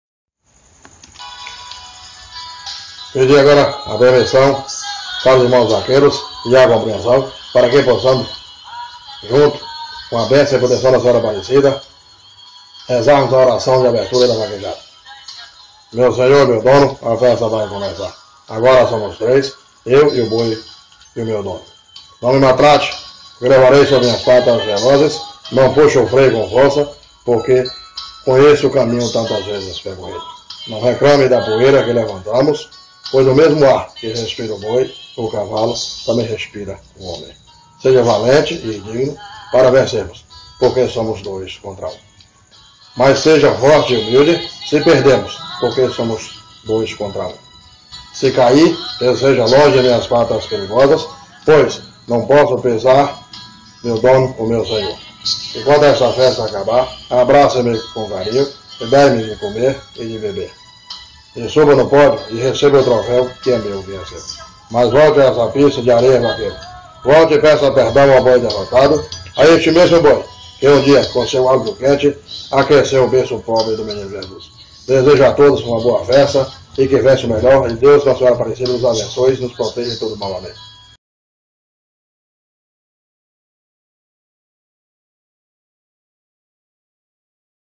ORAÇÃO DE ABERTURA DAS VAQUEJADAS DO NOSSO SERTÃO BRAAILEIRO!!!
NARRAÇÃO E LOCUÇÃO: DO AMIGO GORDO LOCUTOR!!!...